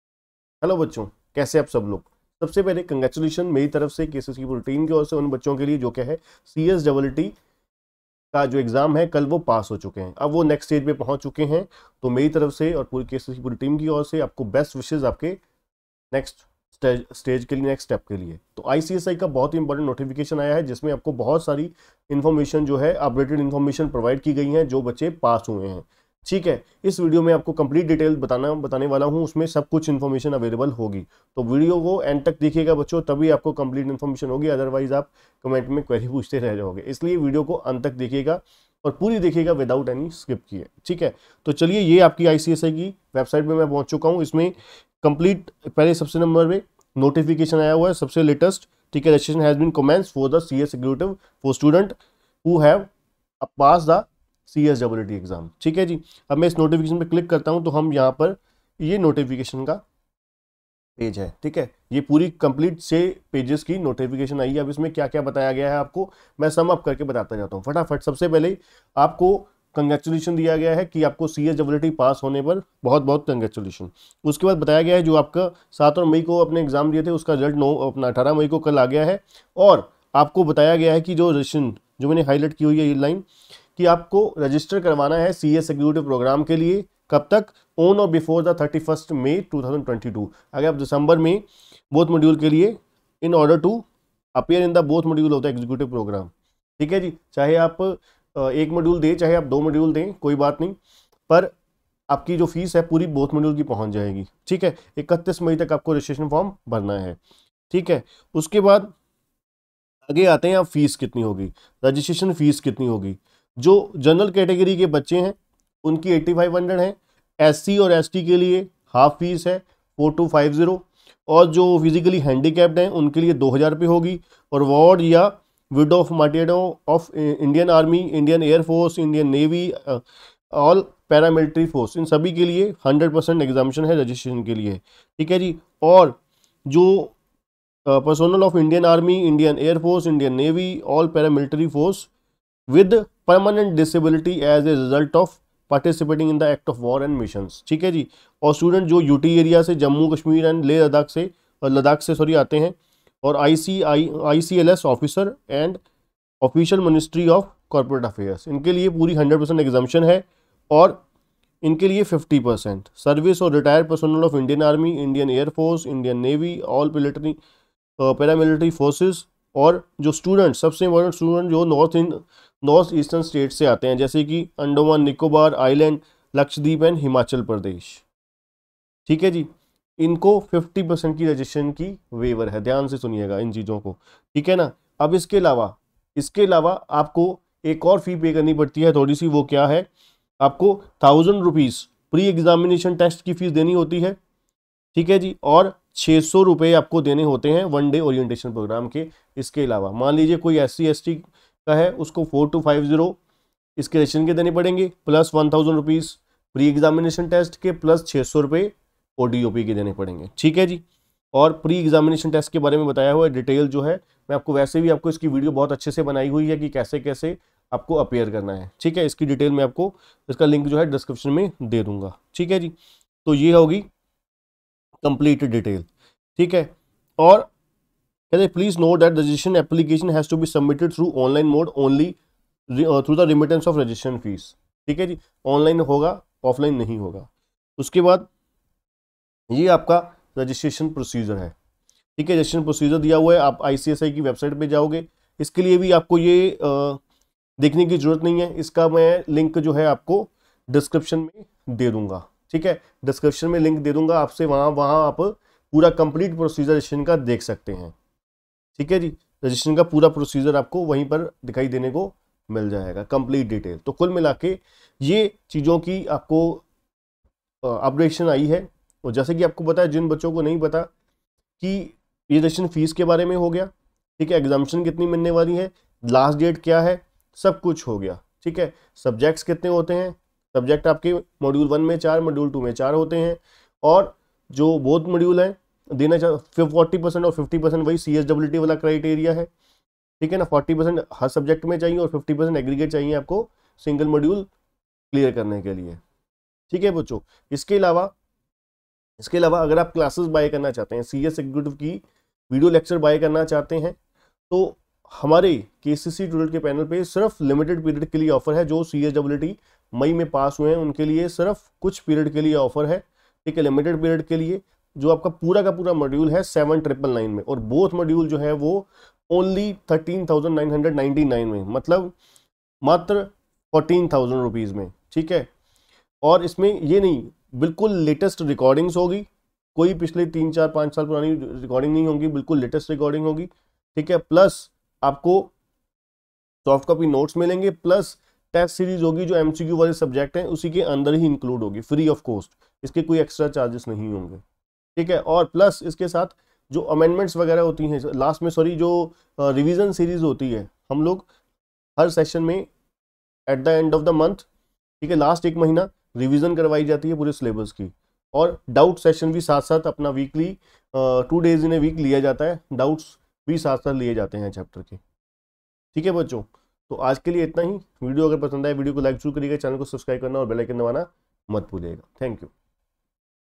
ICSI Important Announcement for CSEET May 2022 Passed Students ! CS Executive December 2022 Exams
Browse the full video and know How to fill CS Executive Registration Form after CSEET exam passed.

To Buy , Call or WA @ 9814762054

   Content of the Video:
    ✔  ICSI Very Important announcement for CSEET May 2022 Passed Students
   ✔ CS Executive Registration Procedure for December 2022 exams
   ✔ CS Executive Registration Fees for Dec 2022 exams
   ✔ Important news for CS EET Passed Students